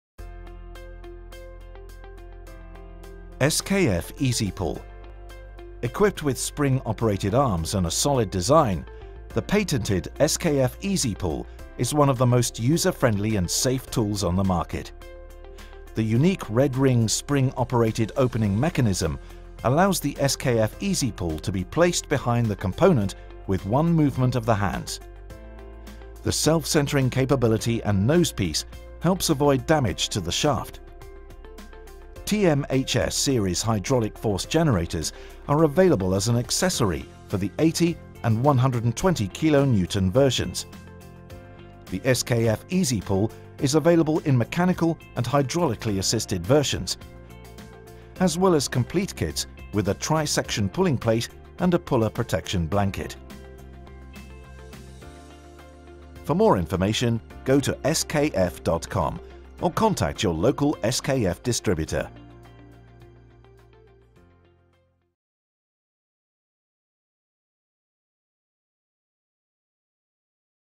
SKF EasyPull Bearing Pullers l SLS Partner SKF
Safe and simple bearing dismounting.
Equipped with spring-operated arms and a solid design, the SKF EasyPull is one of the most user-friendly and safe tools on the market. Ergonomically designed, the spring-operated arms enable the user to position the puller behind the component with just one movement.